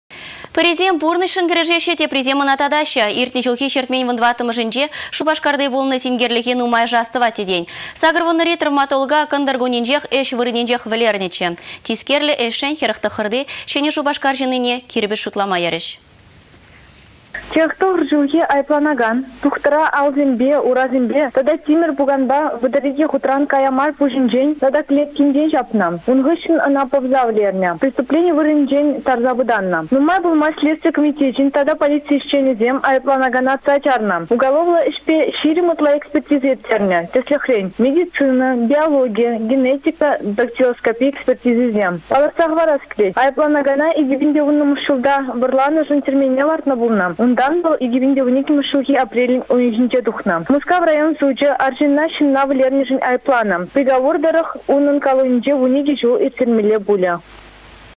«Кирпĕч шутлама каять»
Пĕрисем пурнăçшăн кĕрешеççĕ, теприсем ăна татаççĕ. Иртнĕ çулхи çĕртмен 14-мĕшĕнче Шупашкарти пулнă синкерлĕхе нумайăшĕ ас тăвать те, тен. 80-ри травматолога кăнтăр кунĕнчех, ĕç вырăнĕнчех вĕлернĕччĕ. Тискерлĕ ĕçшĕн 49-ти Çĕнĕ Шупашкар çыннине кирпĕч шутлама ярĕç.